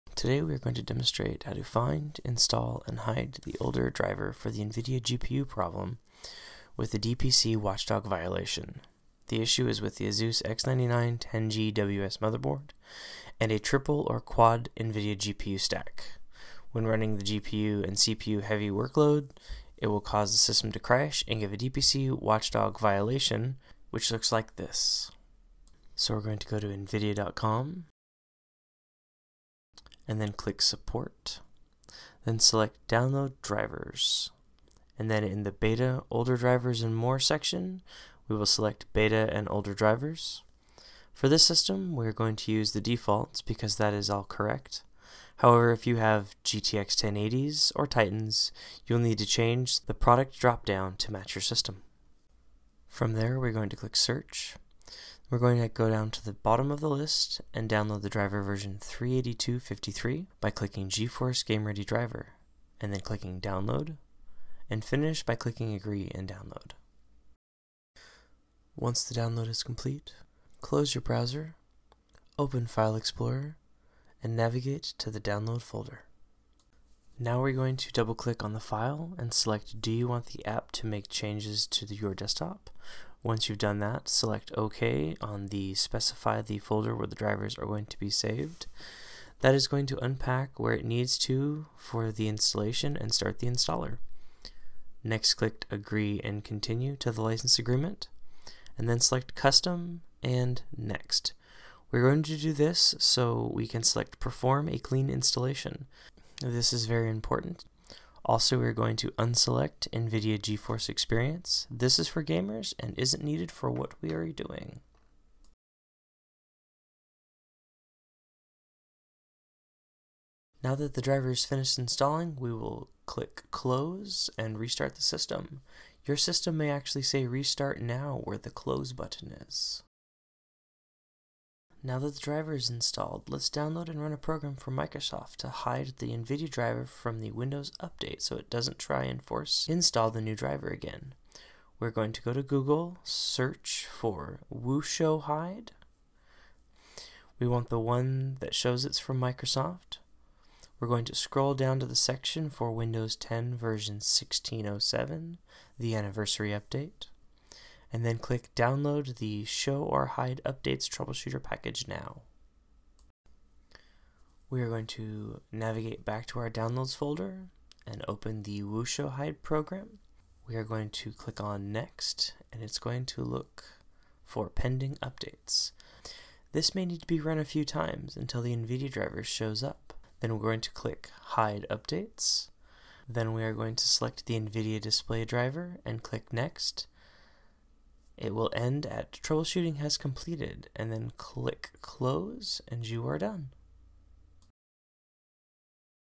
How to Fix DPC Watchdog Violation BSOD on Asus X99E-10G WS
Here at Puget Systems, we have been seeing an issue with the Asus X99E-10G WS motherboard and other boards which use PLX chips to enable triple or quad NVIDIA graphics card configurations. When running a workload that is heavy on both the CPU and GPUs, this hardware combination can crash and give the DPC_WATCHDOG_VIOLATION error (blue screen of death). This is an instructional video that outlines a work around we have found to resolve this issue.